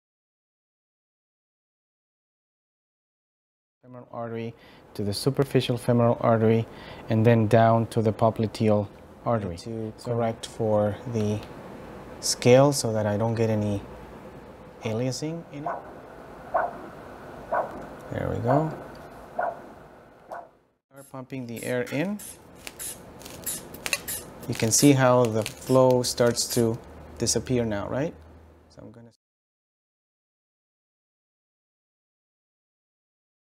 Lower Extremity Arterial Doppler with Ankle-Brachial Index (Trailer)
Lower Extremity Arterial Doppler

What does this video include?
1. Anatomy review of the lower extremity arteries using props on a real patient
2. Step-by-step and detailed explanation of how to scan the lower extremity arteries using ultrasound on a normal patient
3. Example of a protocol to scan the lower extremity arteries including how to obtain the ankle-brachial index
4. Technical tips
5. Summary of important points

This video does not contain plethysmography

Objectives

After viewing this video, the learner will be able to:
• List lower extremity arteries that can be scanned with ultrasound
• Know basic scanning techniques to image the lower extremity arteries
• Describe the normal 2D sonographic features of the lower extremity arteries
• Describe the normal Doppler sonographic features of the lower extremity arteries
• Use the ultrasound protocol example given to scan the lower extremity arteries